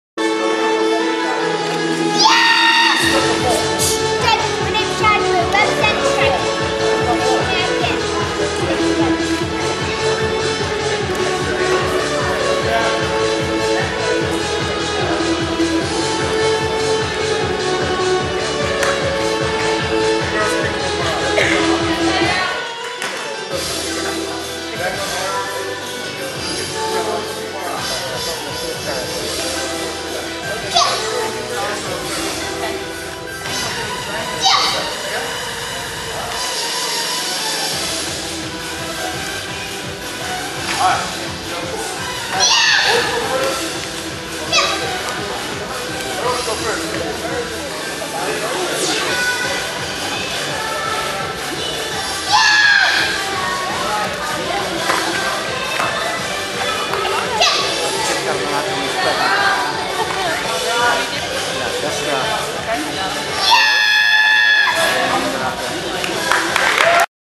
2009 Southern Open – Musical Forms Shai DeBroux
2009 Southern Open Friday, October 2, 2009 – Musical Forms Shai DeBroux.

Video courtesy of Webs By MKS, LLC.